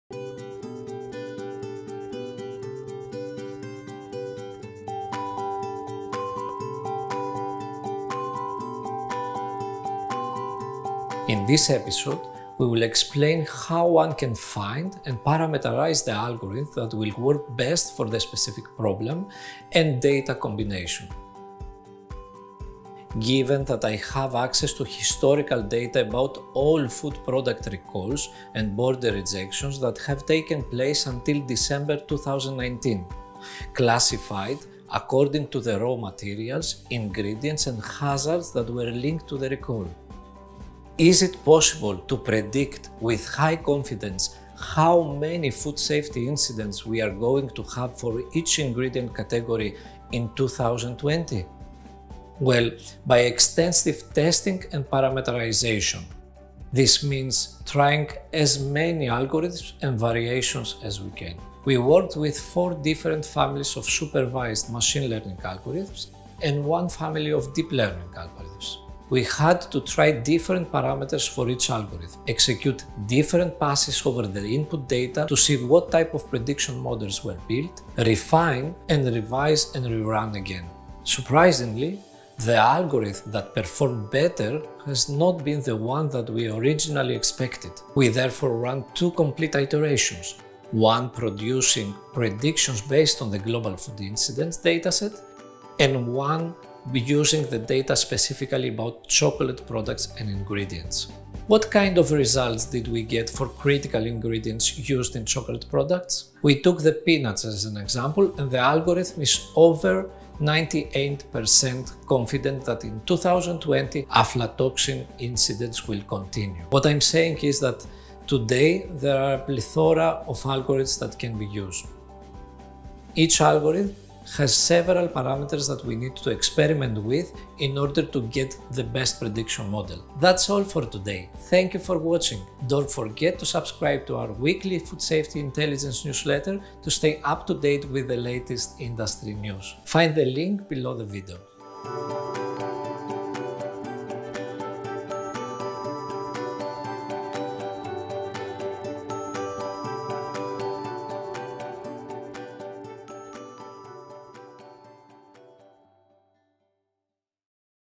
Episode 3: How to select the right algorithm?
Technology and predictive analytics are critical for the future of food safety.
Determining which predictive analytics techniques work best for a particular challenge is the key to making the most of a predictive analytics solution.
In this episode of our mini-video series, Giannis Stoitsis, CTO, and partner at Agroknow, explains how we can find and parameterize the algorithm that will work best for a specific problem and data combination.